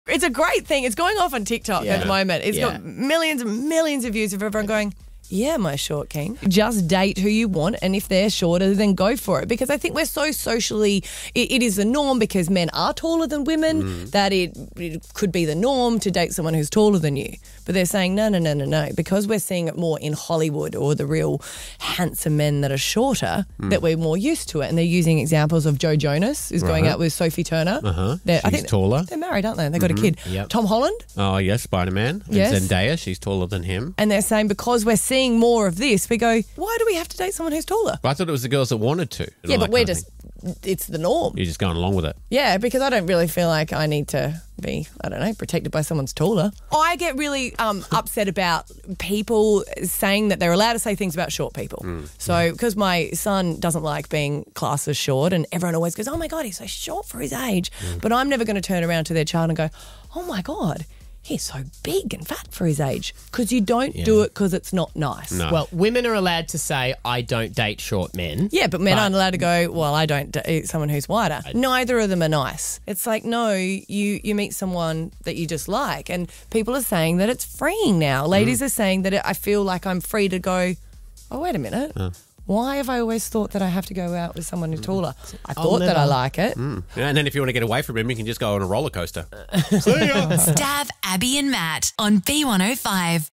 Short King Spring Is Here and Abby's Glad | B105
It's officially Short King Spring 👑
Abby's relieved that we're finally stopping judging people by their height 🥲